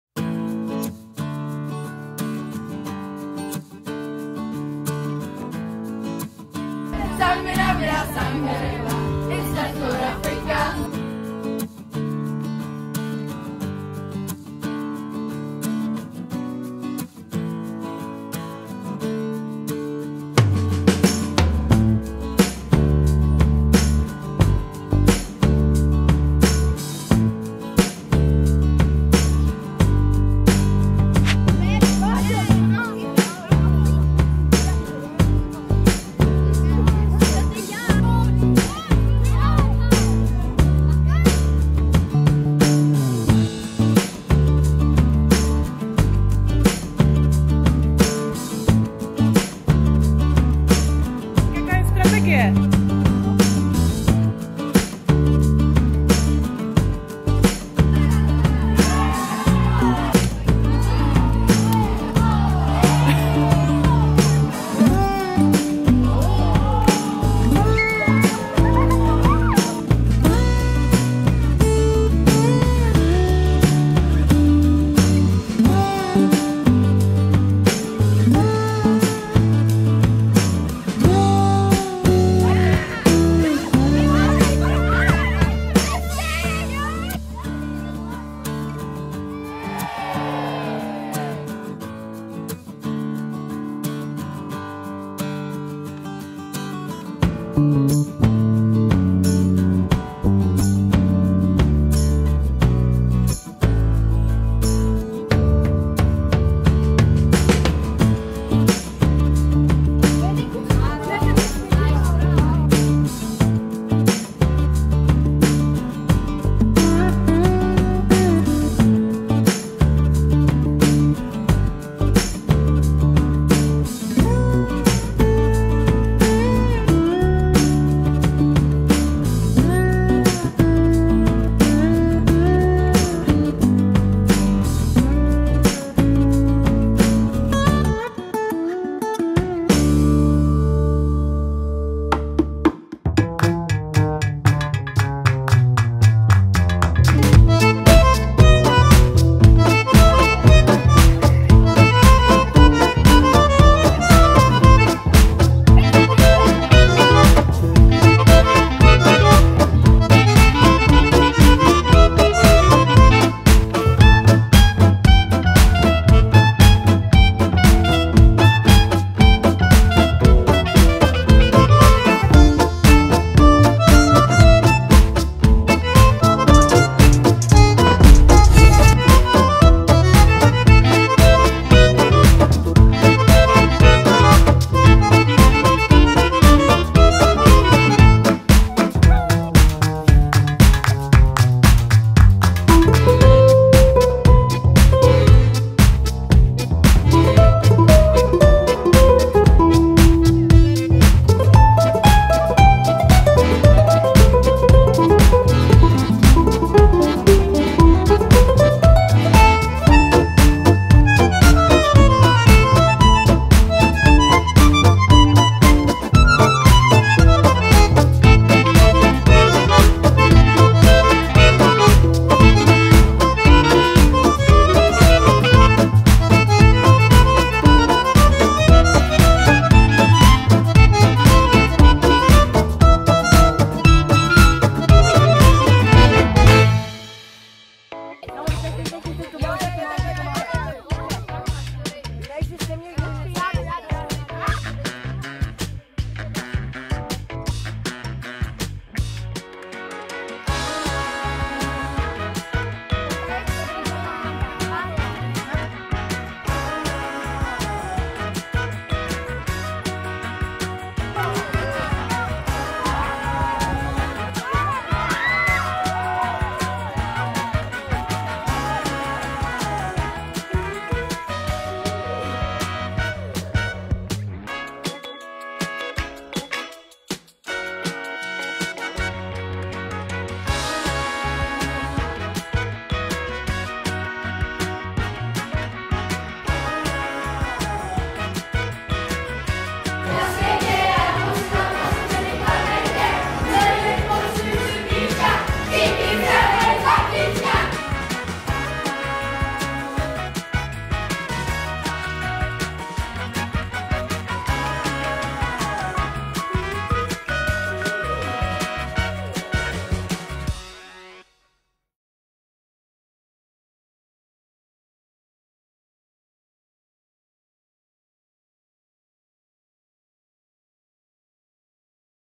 Begegnungsfahrt der 7. Klassen 2022
Für unsere 7. Klassen ging es in diesem Jahr zur Begegnungsfahrt nach Plánice.

Náš 7. stupeň se tento rok vydal na svůj stmelovací výlet do Plánice.